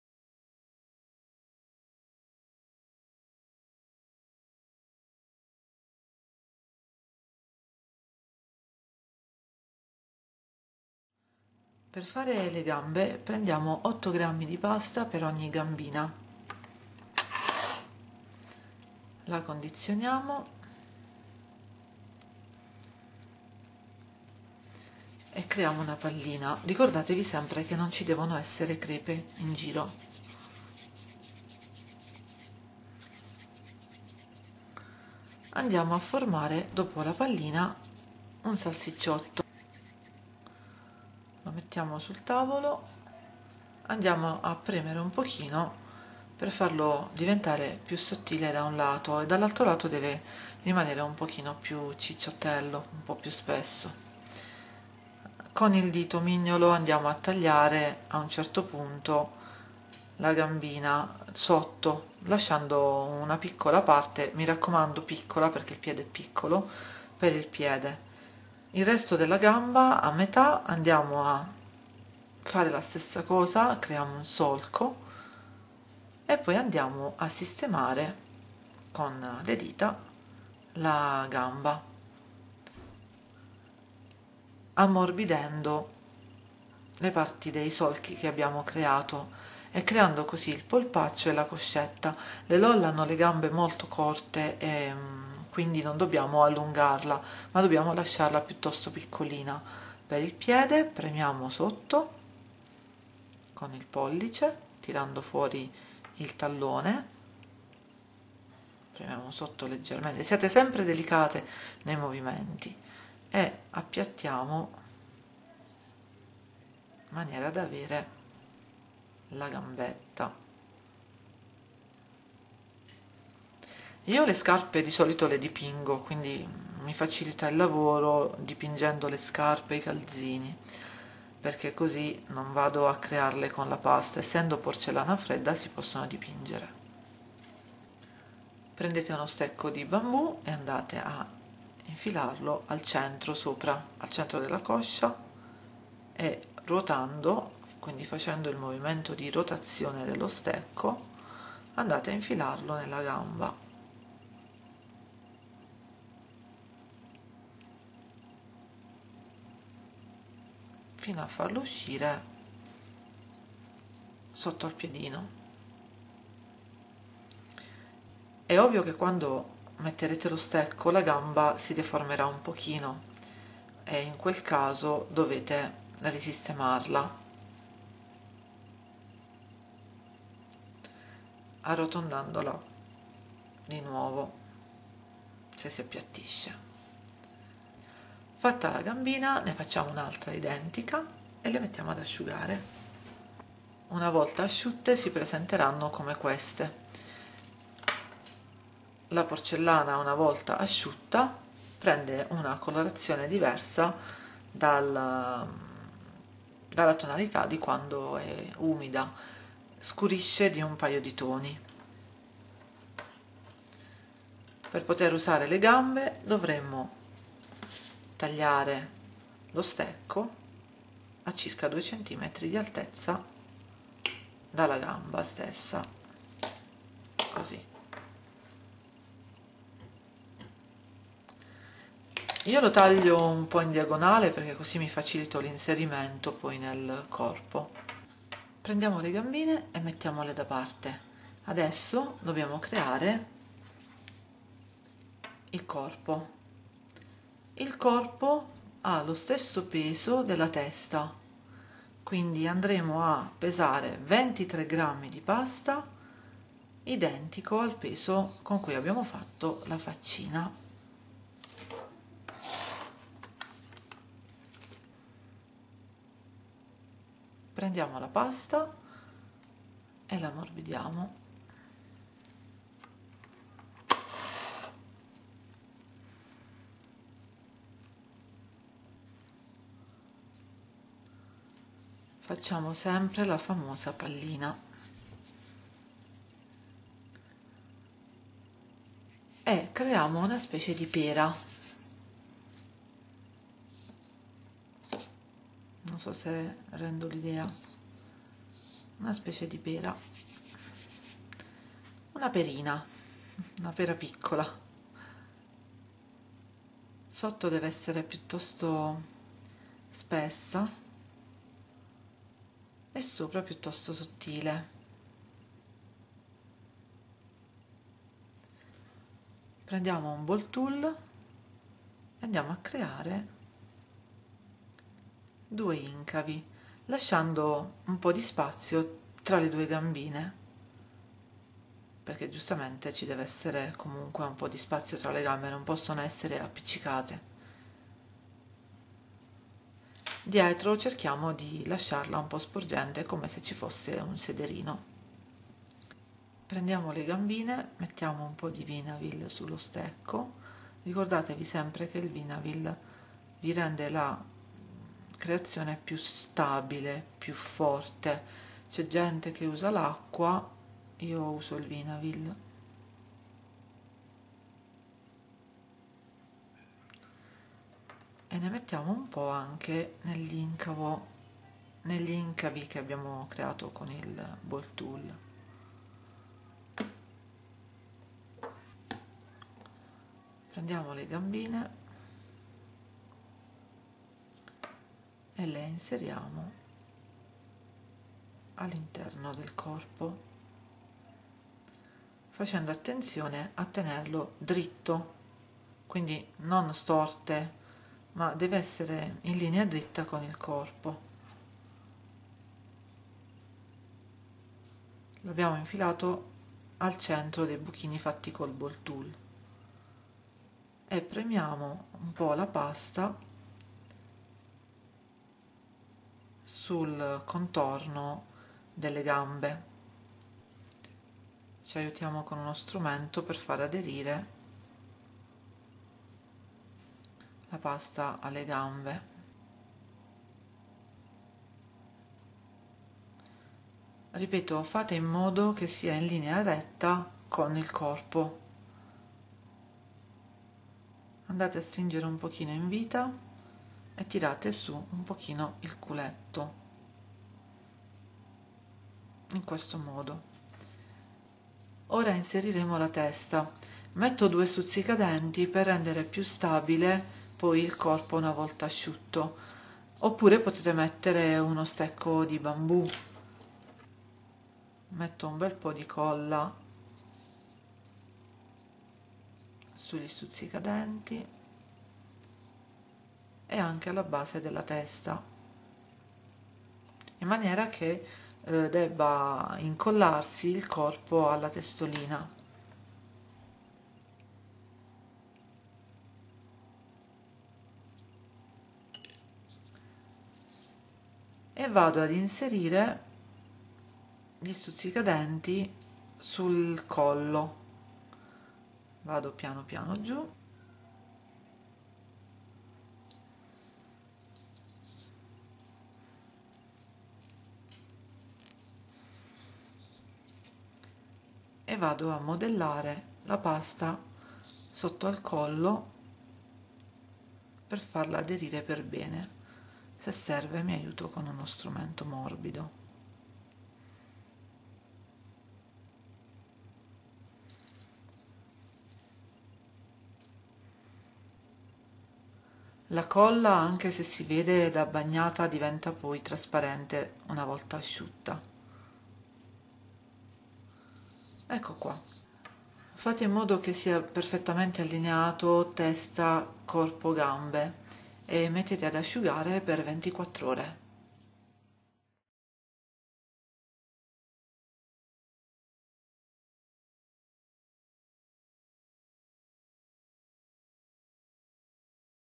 Tutorial LOL surprise parte 2 GAMBE E CORPO - Leg and body LOL surprise tutorial
Parte 2: il gambe e corpo di una LOL surprise realizzato in porcellanafredda.

Seguiranno altri video per il completamento della bambolina :)
Utile per creare caketopper o decorazioni per torte